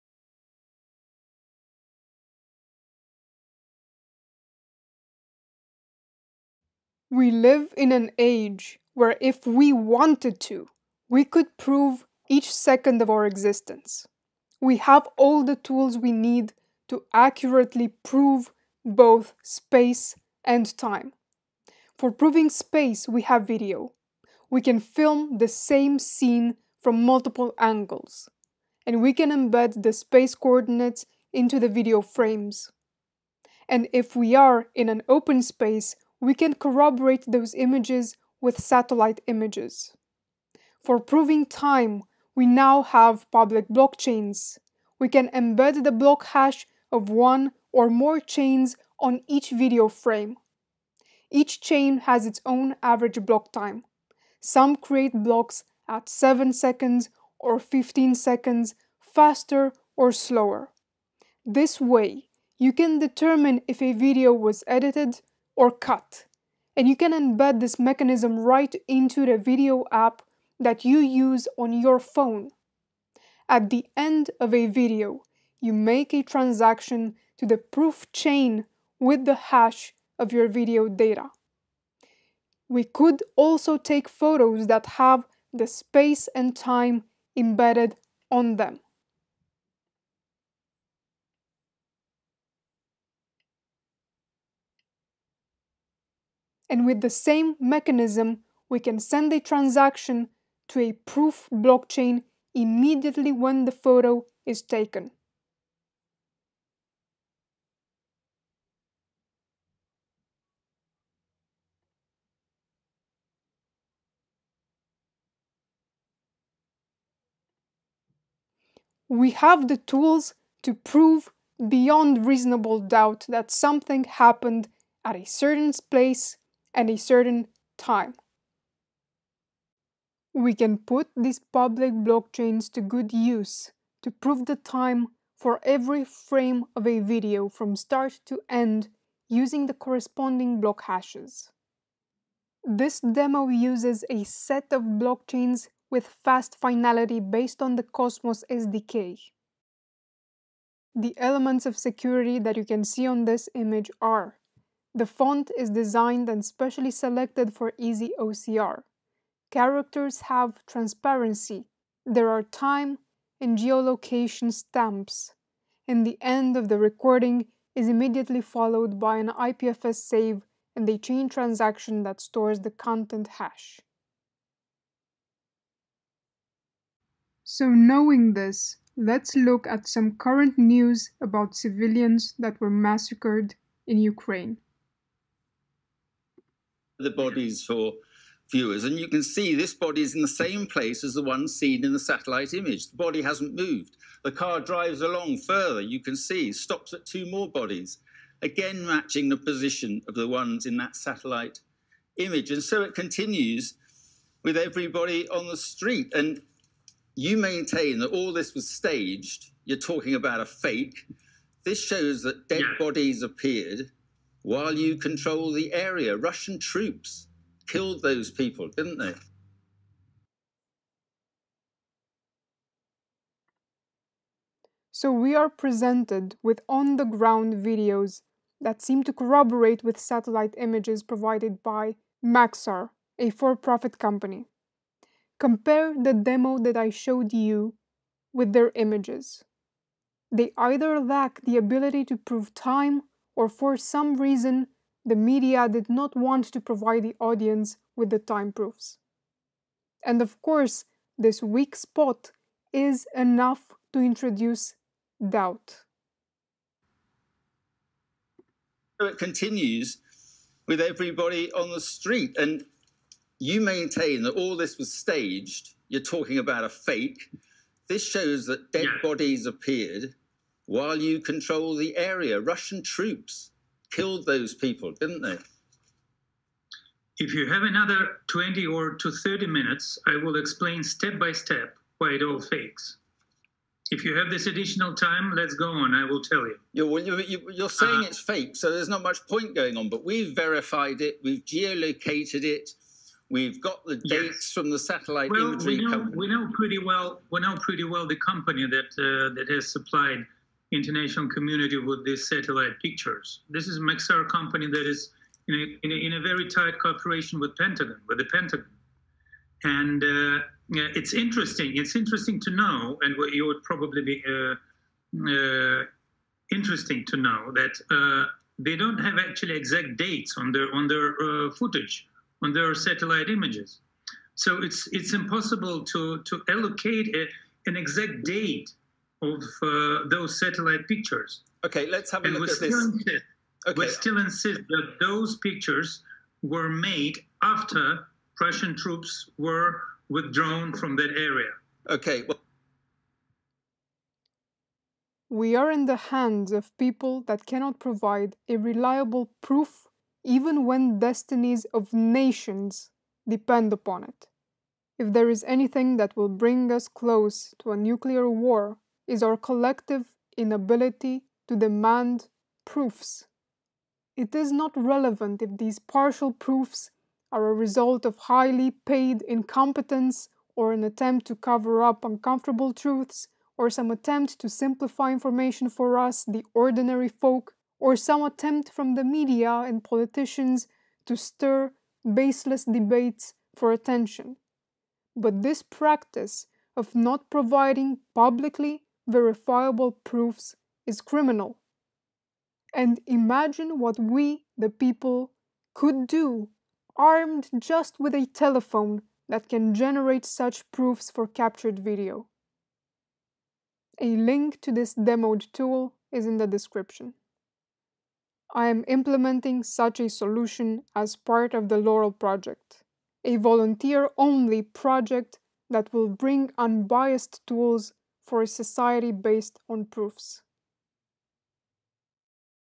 Provable News - Make Proofs Not War
For proving space, we have video. We can film the same scene from multiple angles. We can embed the space coordinates into the video frames. And if we are in an open space, we can corroborate those images with satellite images.
For proving time, we now have public blockchains.